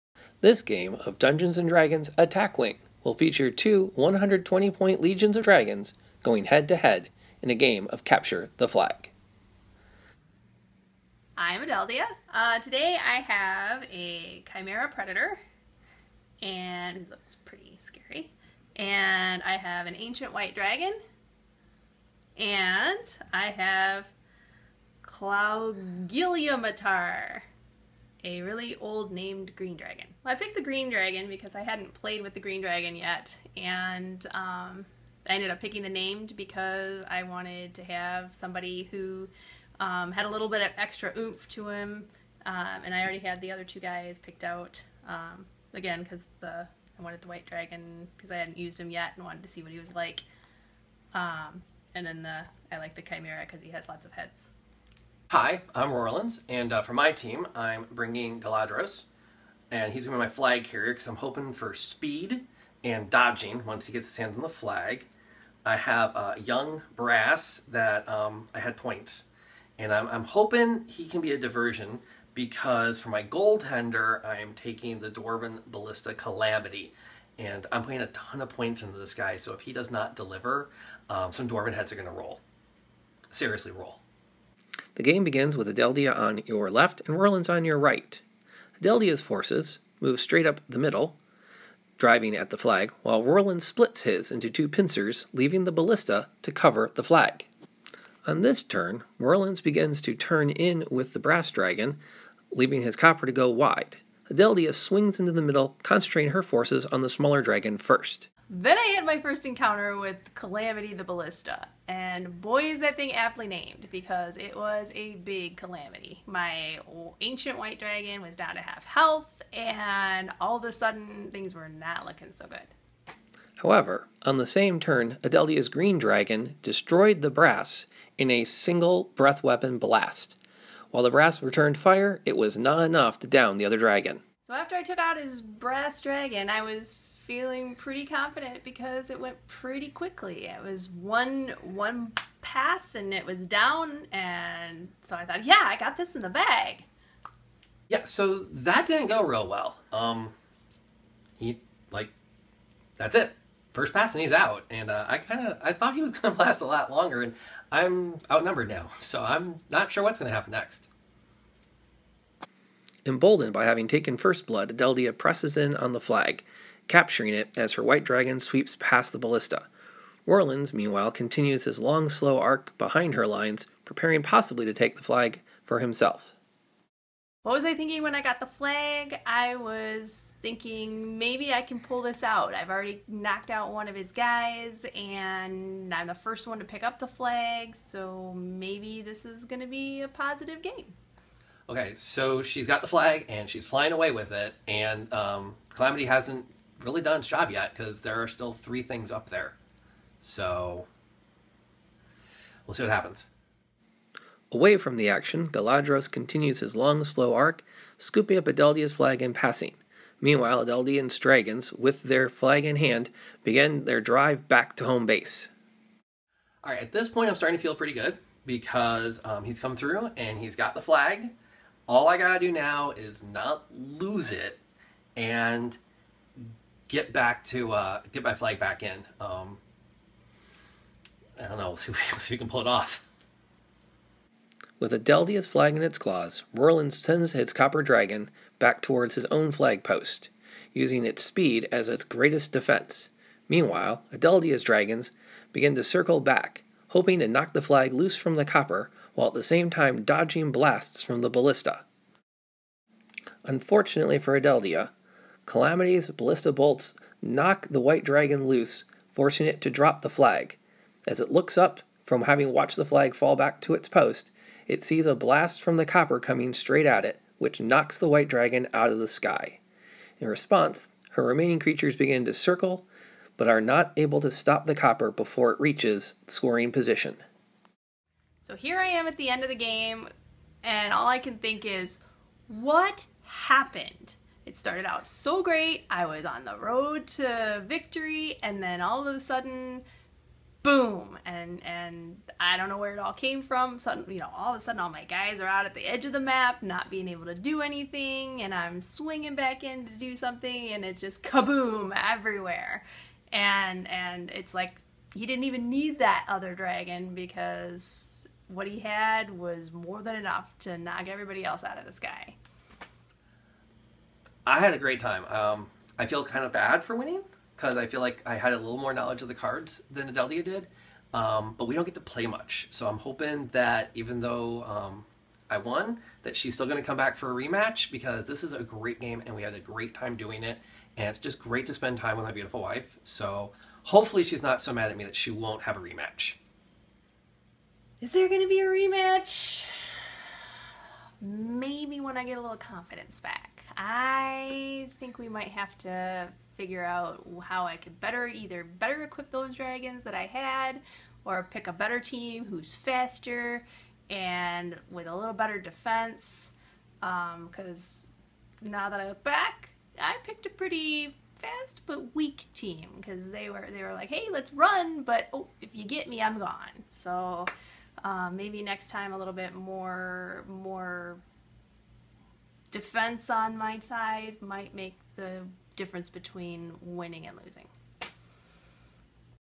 Attack wing: CtF
Capture the flag with Rorlins and Adeldia playing 120 point legions.